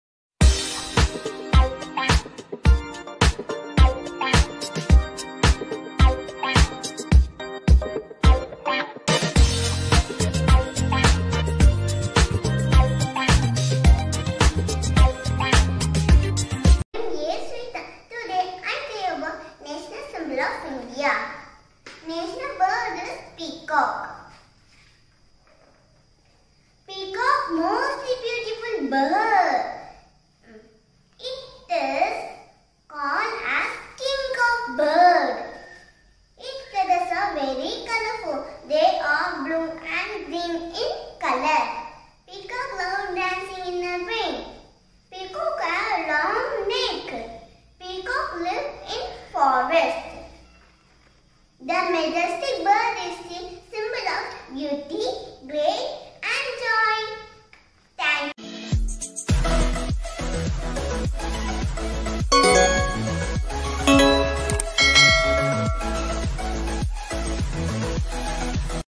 Award winning speech 2021 peacock speech (National symbol)
Hi everyone it's me A.Suvitha welcome to Star Kidz Today in this video I am going to tell you about peacock (Award winning speech for kids)